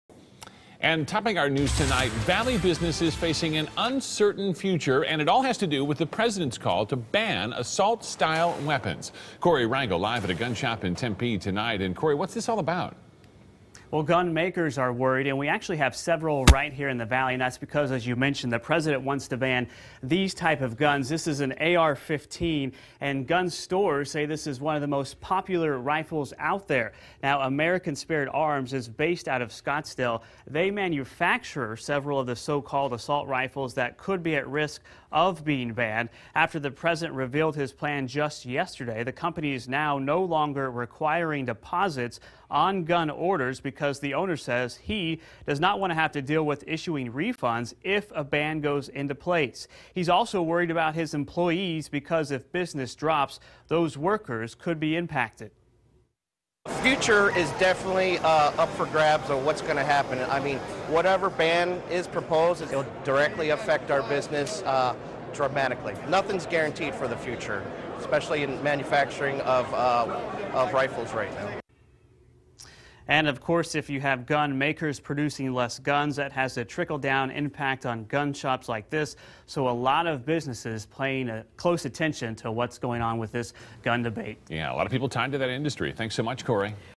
Valley business facing uncertain future
American Spirit Arms delaying new orders.